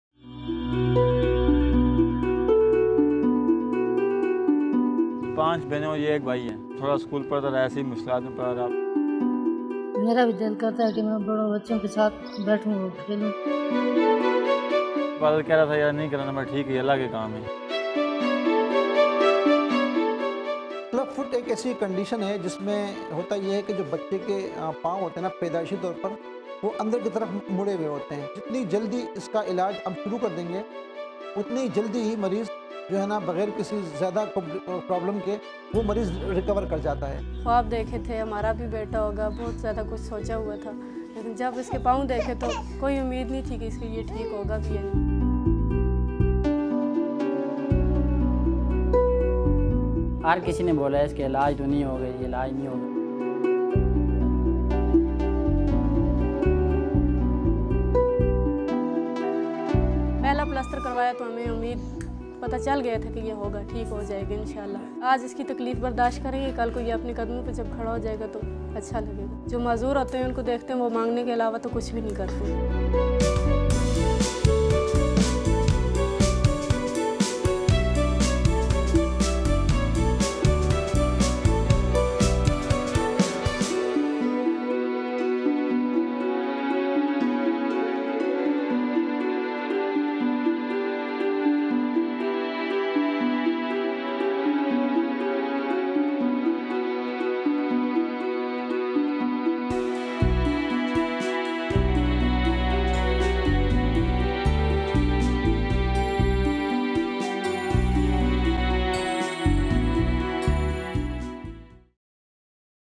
What if you could change Ayaan’s life?
Clubfoot is a 100% curable congenital disorder if treated on time. But lack of access and costs of treatments remain strong obstacles for Pakistan families in needs. Discover their stories and how you can contribute to their relief